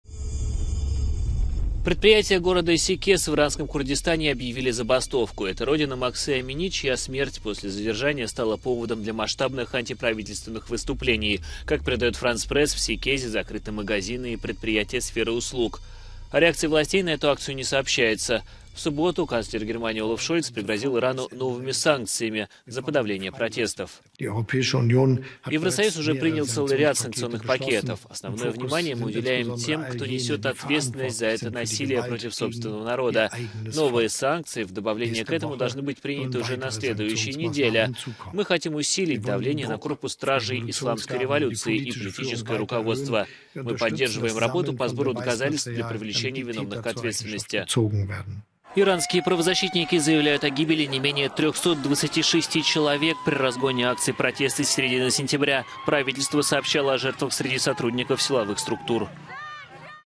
Забастовка на родине Махсы Амини в иранском Курдистане
Предприятия города Секкез объявили акцию протеста в память о землячке, смерть которой после задержания спровоцировала масштабную антиправительственную кампанию.